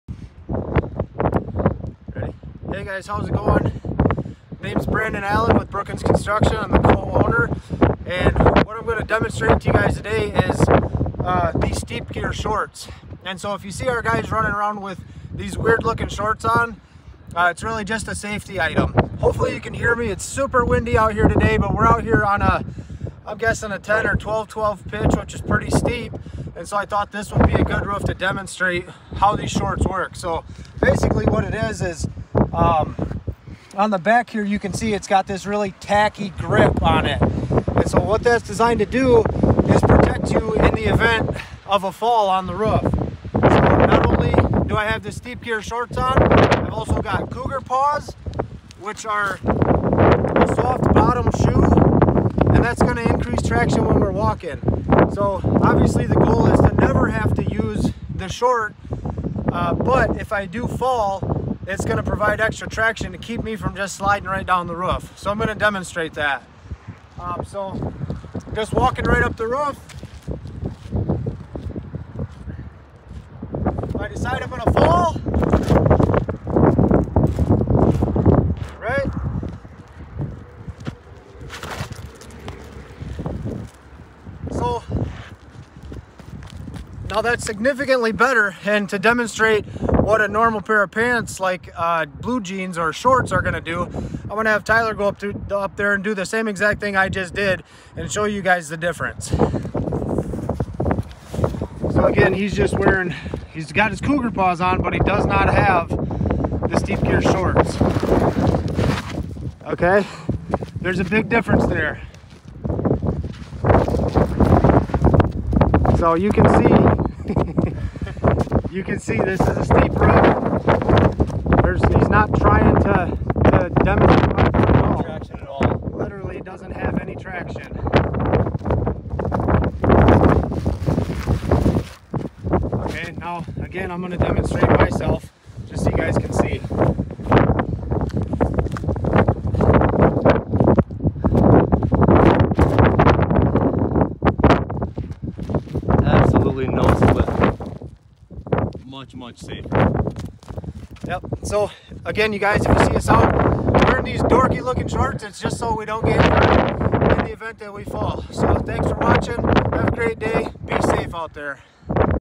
Why are we wearing these funny looking shorts?
Safety is top priority for us that's why we use the SteepGear Roof Safety Anti-Slip Shorts.